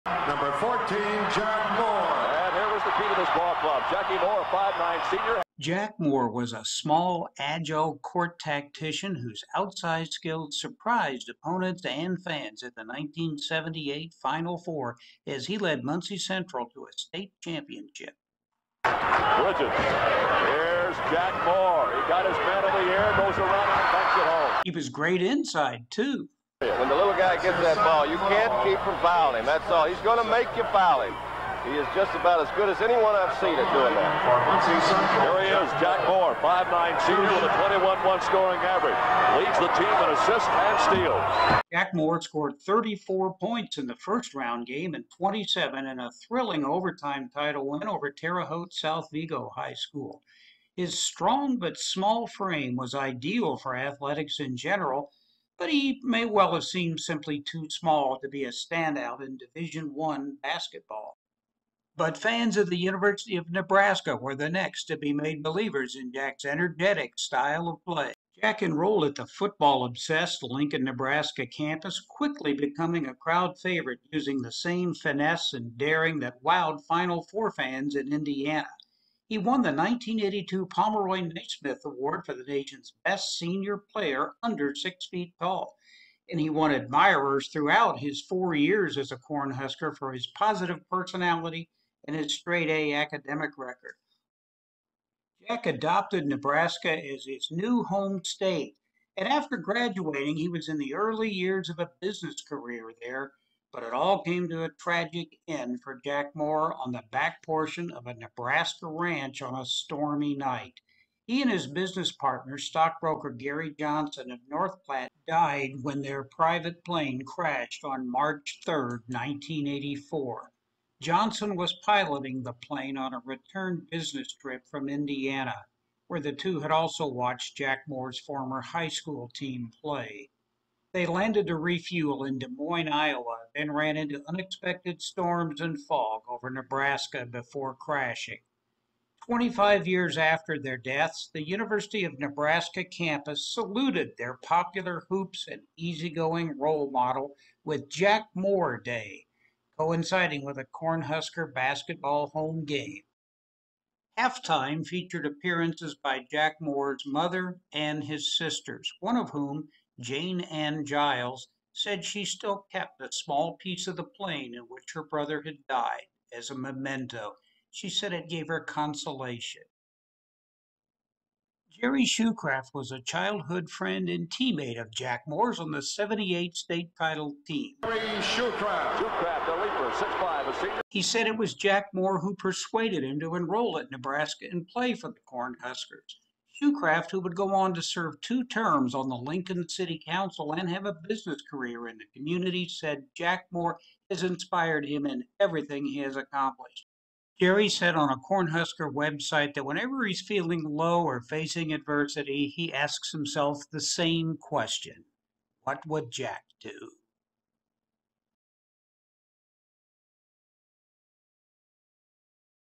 Jack Moore
This is a segment of a YouTube being made about Indiana high school basketball stars, particularly at the state's Final 4, who died young.